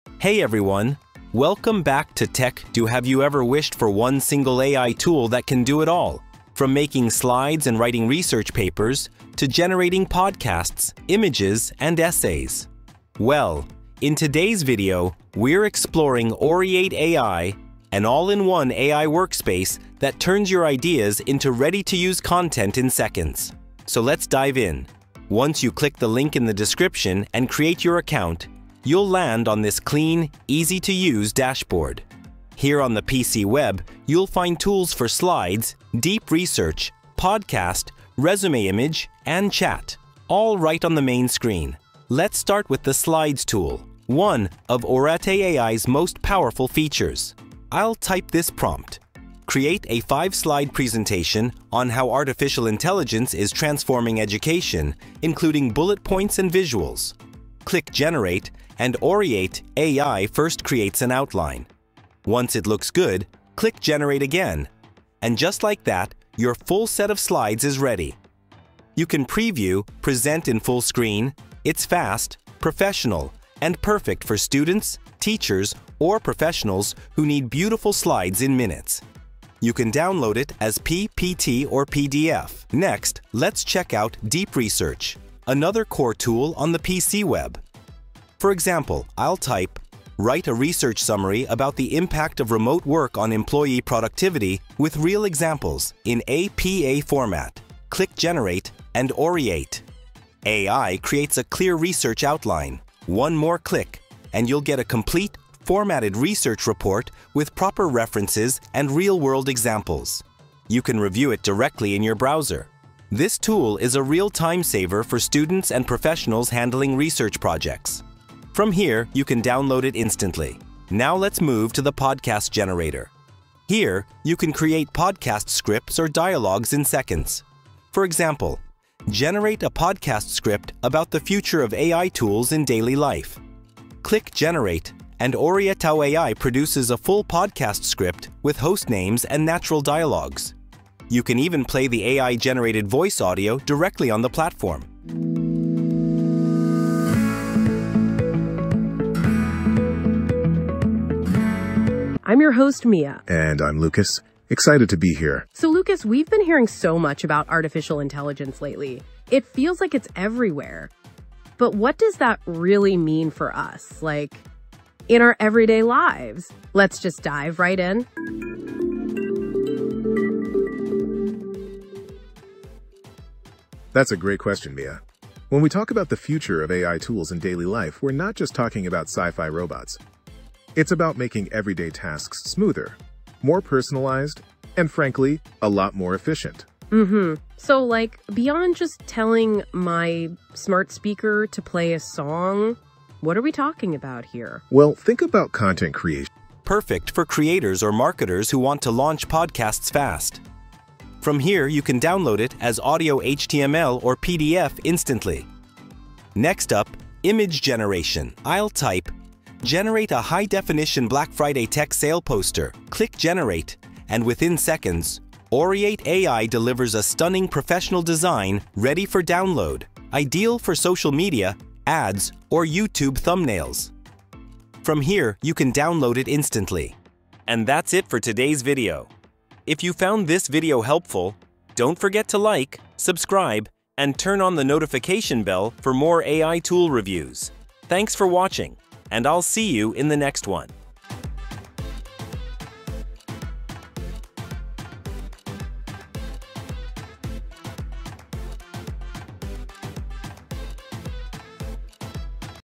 Oreate AI – The Ultimate All-in-One AI Tool for 2025!
Meet Oreate AI, your all-in-one AI workspace that helps you create everything — from slides and research papers to podcasts, essays, and images — in just seconds! Try it

In this video, I’ll walk you through Oreate AI’s main tools:
💡 Slides Generator – Instantly create stunning, editable presentations.
📚 Deep Research – Generate formatted research papers with references.
🎙 Podcast Creator – Build full scripts and AI-generated audio.
🖼 AI Image Generator – Design posters, banners, and thumbnails in seconds.
🧠 Essay Writer & Humanizer – Write or rewrite essays that sound 100% human.

Whether you’re a student, content creator, or professional, Oreate AI saves you hours of work with just a few clicks.